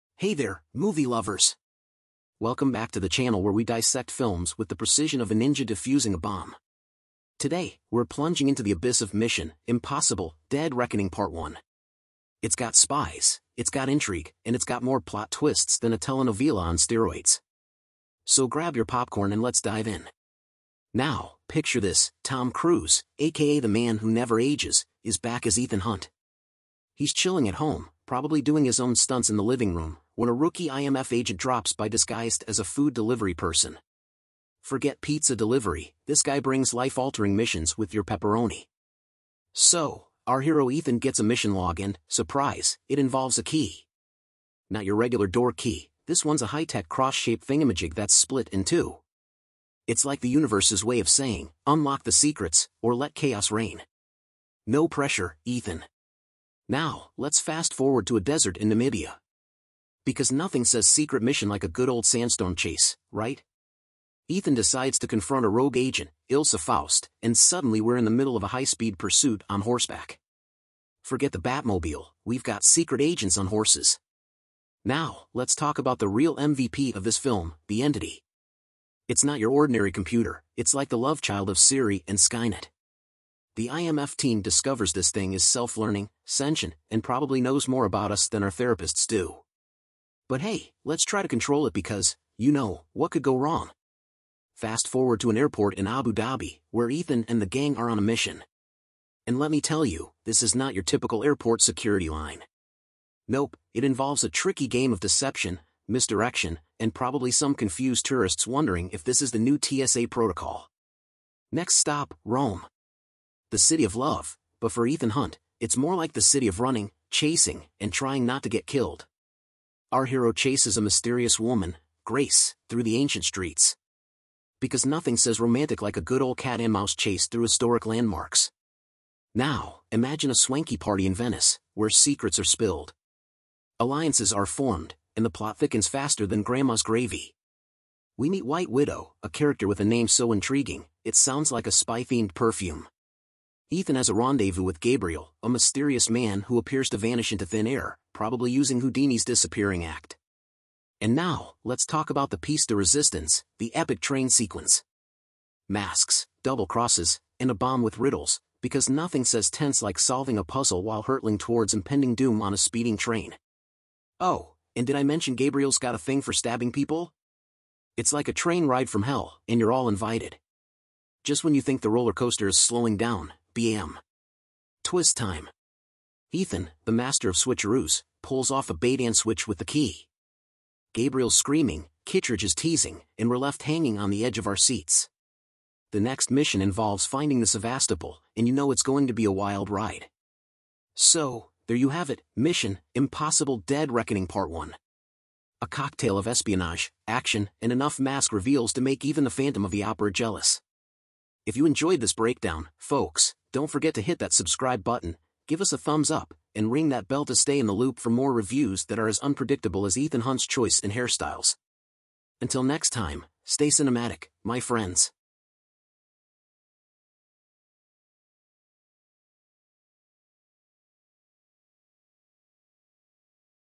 Mission Impossible:Dead reckoning Part 1 Review
In the highly anticipated movie, Mission: Impossible - Dead Reckoning, Part One, Ethan Hunt (played by Tom Cruise) and his IMF team embark on their most dangerous mission yet. Their objective is to track down a terrifying new weapon that poses a threat to humanity. With the future of the world hanging in the balance, Ethan and his team must race against time to prevent the weapon from falling into the wrong hands. As they face off against a powerful enemy and confront their own dark pasts, the stakes have never been higher.